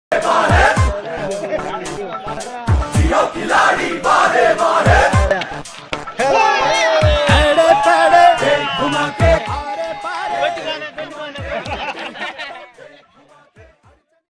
Qasim Saraj Soomro Ki Bolling |MPA PPP
قاسم سراج سومرو صاحب(MPA) دوست محمد راہموں کو امر جگديش ملانی کرکٹ گراؤنڈ مٹھی میں بالنگ کرواتے ہوئے،
دوست محمد راہموں کا چھکا.